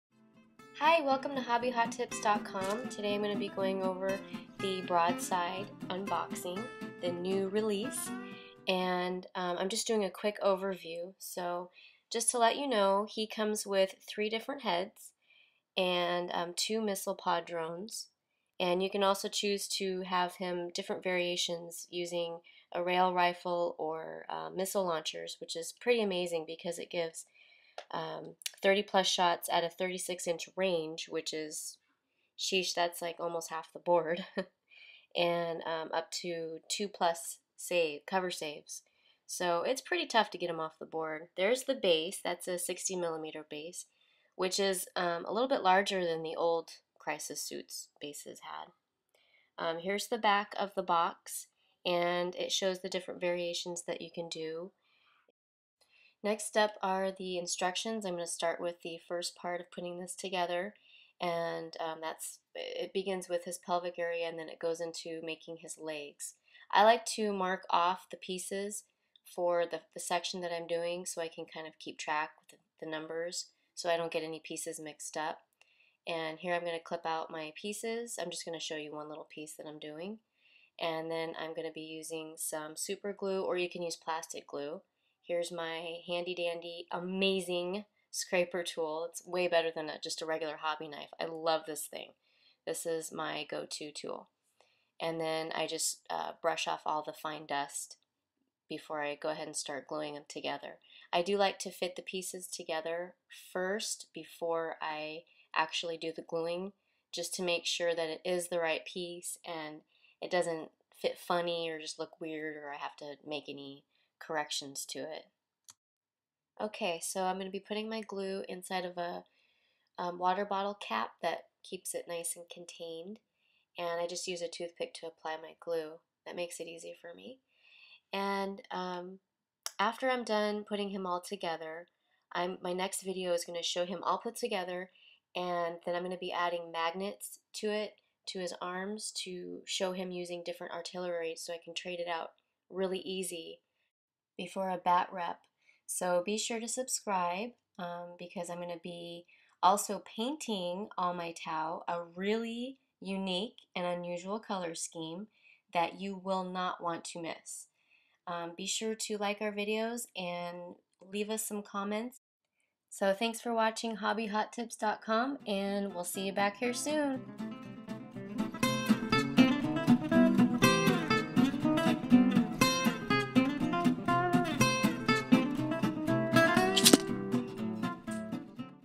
miniGIRL #01: NEW RELEASE TAU - Broadside Battlesuit XV88 Unboxing! 2013
1st vid journey of unboxing Tau, painting, gaming, to doing a batrep.